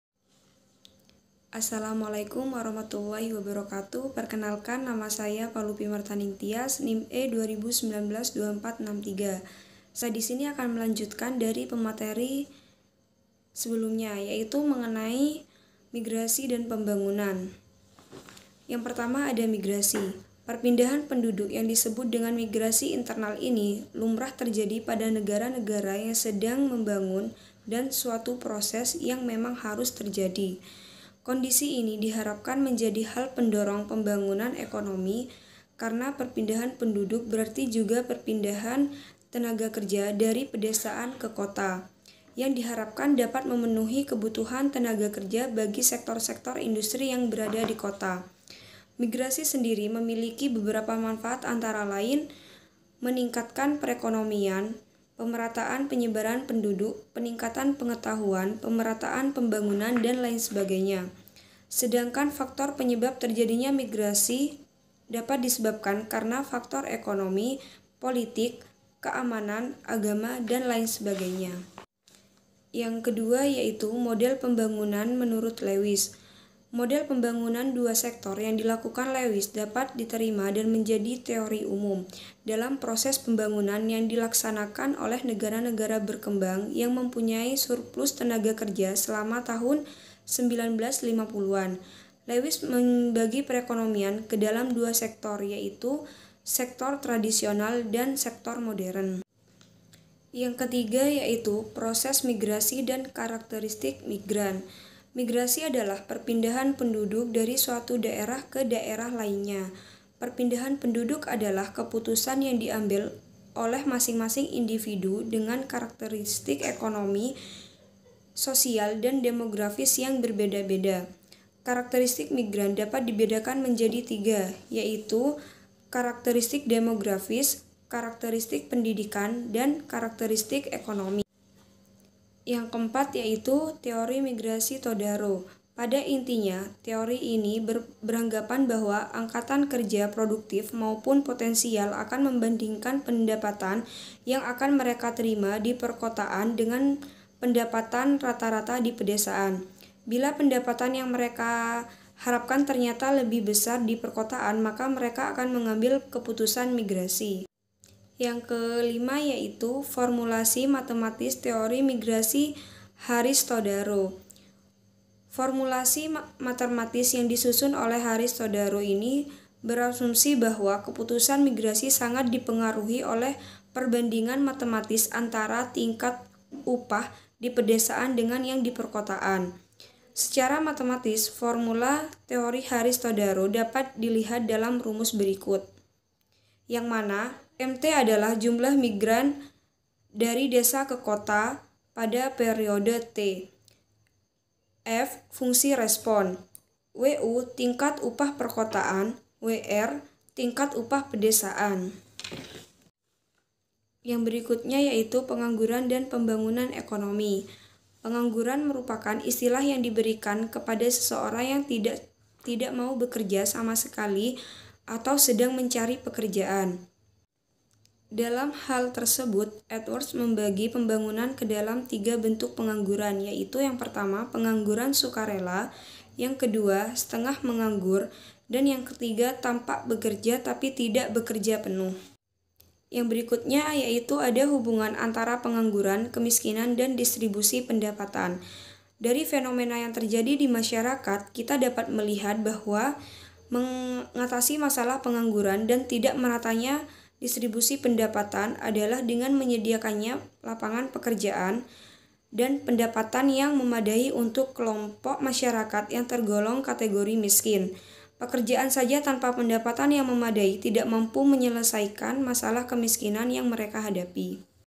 part 2|Klp 8 Kependudukan dan Tenaga Kerja|Palupi Martaning Tyas(2463)|ES10
Migrasi dan Pembangunan
1. Migrasi
Perpindahan penduduk yang disebut dengan migrasi internal ini, lumrah terjadi pada negara-negara yang sedang membangun dan suatu proses yang memang harus terjadi. Kondisi ini diharapkan menjadi hal mendorong pembangunan ekonomi, karena perpindahan penduduk berarti juga perpindahan tenaga kerja dari pedesaan ke kota-kota, yang diharapkan dapat memenuhi kebutuhan tenaga kerja bagi sektor-sektor industri di perkotaan.
Mingrasi sendiri memiliki beberapa manfaat  antara lain: meningkatkan perekonomian, pemerataan penyebaran penduduk, peningkatan pengetahuan, pemerataan pembangunan, dsb. Sedangkan faktor penyebab terjadinya migrasi dapat disebabkan karena faktor ekonomi, politik, keamanan, agama dan sebagainya.
2. Model Pembangunan Menurut Lewis
Model pembangunan dua sektor yang dikemukakan Lewis dapat diterima dan menjadi teori umum (general theory) dalam proses pembangunan yang dilaksanakan oleh negara-negara berkembang yang mempunyai surplus tenaga kerja selama tahun 1950-an.
Dua sektor menurur lewis :
* Sektor tradisional
* Sektor modern
3. Proses Migrasi dan Karakteristik Migran
Migrasi adalah perpindahan penduduk dari suatu daerah ke daerah lainnya. Perpindahan penduduk adalah keputusan yang diambil oleh masingmasing individu dengan karakteristik ekonomi, sosial, dan demografi yang berbeda-beda.
Karakteristik migrasi :
* Demografis
* pendidikan
* Ekonomi
4. Teori Migrasi Todaro
Pada intinya teori ini beranggapan bahwa angkatan kerja produktif maupun potensial akan membandingkan pendapatan yang akan mereka terima di perkotaan dengan pendapatan ratarata di pedesaan. Bila pendapatan yang mereka harapkan ternyata lebih besar di perkotaan, maka mereka akan mengambil keputusan migrasi.
5. Formulasi Matematis Teori Migrasi Haris-Todaro
Formulasi matermatis yang disusun oleh Harris-Todaro ini berasumsi bahwa keputusan migrasi sangat dipengaruhi oleh perbandingan matematis antara tingkat upah di pedesaan dengan yang ada di perkotaan. Secara matematis, formula teori Harris Todaro dapat dilihat dalam rumus berikut :
Mt = f (Wu-Wr)
Mt = jumlah migran dari desa ke kota pada periode t
F = fungsi respon
Wu= tingkat upah perkotaan
Wr = tingkat upah pedesaan

Pengangguran dan Pembangunan Ekonomi
1. Pengangguran
 Pengangguran merupakan istilah yang di berikan kepada orang yang tidak bekerja sama sekali atau orang yang sedang mencari pekerjaan . Pengangguran juga dapat diartikan sebagai sebuah situasi ketika seseorang tidak memiliki pekerjaan.
3 bentuk pengangguran :
* pengangguran sukarela
* Setengah menganggur
* Tampak bekerja tetapi tidak bekerja penuh
2.  Hubungan antara Pengangguran,Kemiskinan dan Distribusi Pendapatan
Dari fenomena yang terjadi di masyarakat, kita dapat melihat bahwa untuk mengatasi masalah pengangguran dan tidak meratanya distribusi pendapatan, adalah dengan menyediakan lapangan kerja dan pendapatan yang memadai untuk kelompok masyarakat yang tergolong kategori miskin. Pekerjaan saja tanpa pendapatan yang memadai tidak mampu menyelesaikan masalah kemiskinan yang mereka hadapi.